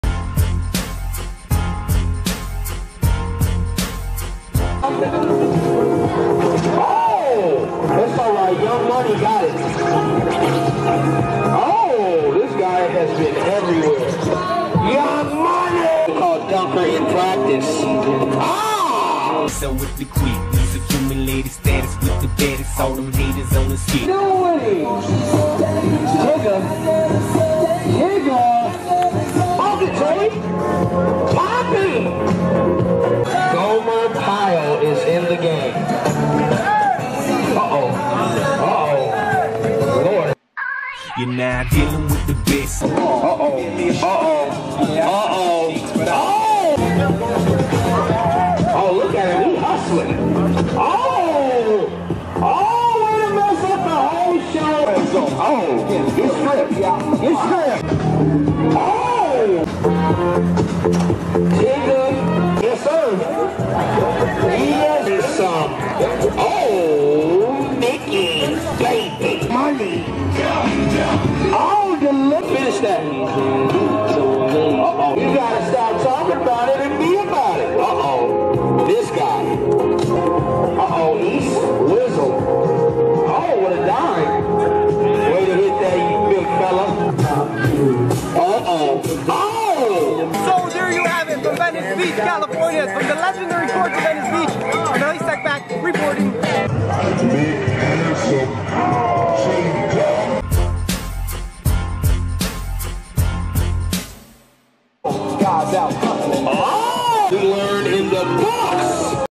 Watch Elie Seckbach Ballin In Venice Beach with NBA Player | EsNews Boxing
Watch ESNEWS Elie Seckbach balling in Venice Beach basketball courts. Looks like Elie Seckbach from ESNEWS is playing with an NBA player. Can you spot this NBA Player? If so, leave your guess below and join the conversation.